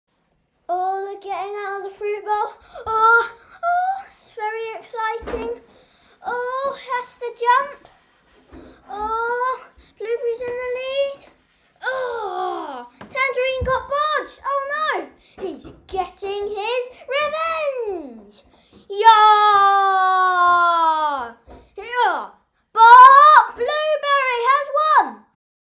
Alex's Fruit Race
See an orange, a temperamental tangerine, a peach and a blueberry compete for the Trophy of the Fruit Kingdom.